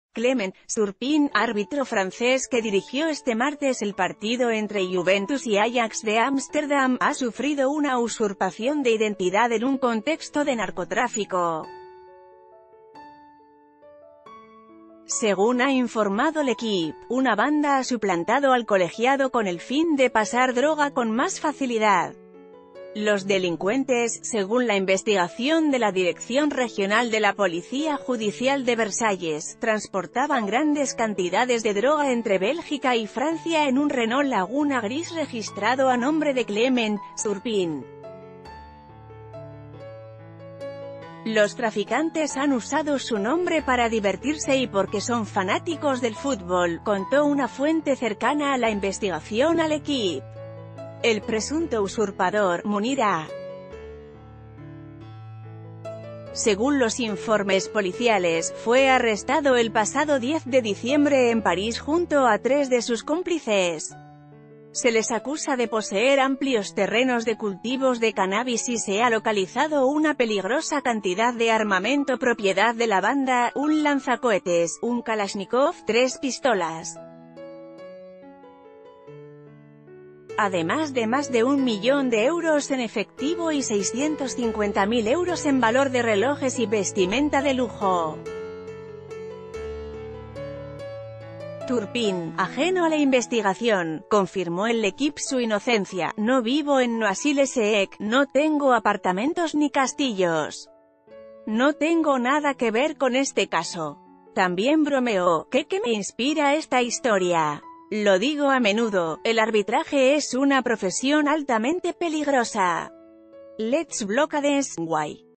Usurpan la identidad del árbitro Turpin para transportar droga
Clement Turpin, árbitro francés que dirigió este martes el partido entre Juventus y Ajax de Ámsterdam, ha sufrido una usurpación de identidad en un contexto de narcotráfico. Según ha informado L'Equipe, una banda ha suplantado al colegiado con el fin de pasar droga con más facilidad. Los delincuente...